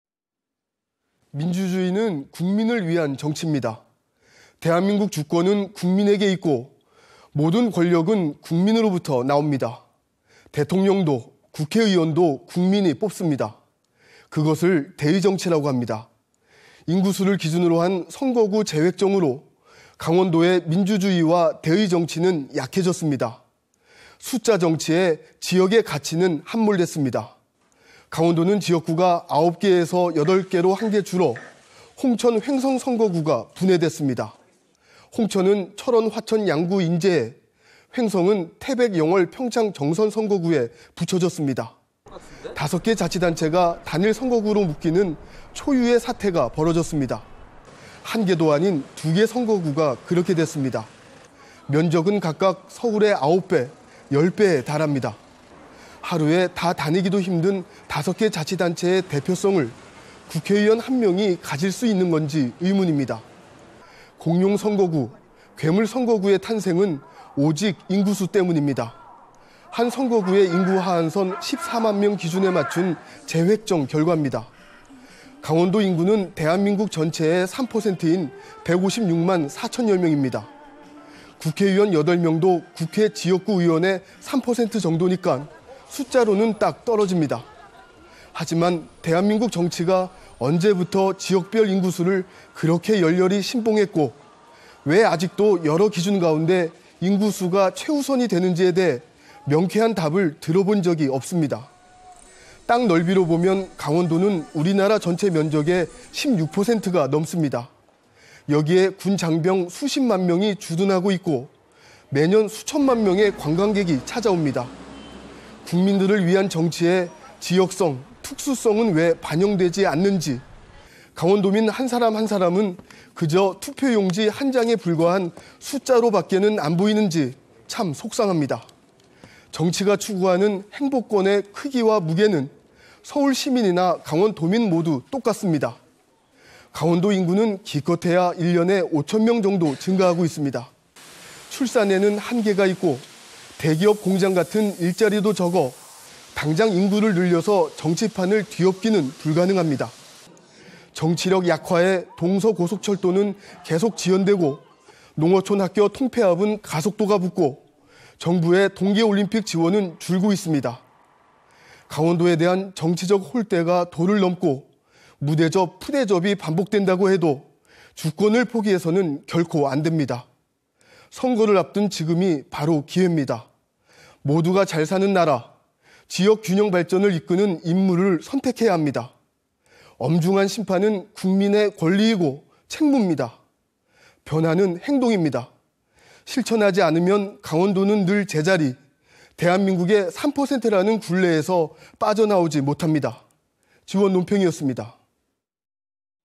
G1논평 - "강원도가 대한민국의 3%로 보이는가"
민주주의는 국민을 위한 정치입니다.

대한민국 주권은 국민에게 있고 모든 권력은 국민으로부터 나옵니다.

대통령도, 국회의원도 국민이 뽑습니다.

그것을 대의정치라고 합니다.

인구 수를 기준으로 한 선거구 재획정으로 강원도의 민주주의와 대의정치는 약해졌습니다.

숫자 정치에 지역의 가치는 함몰됐습니다.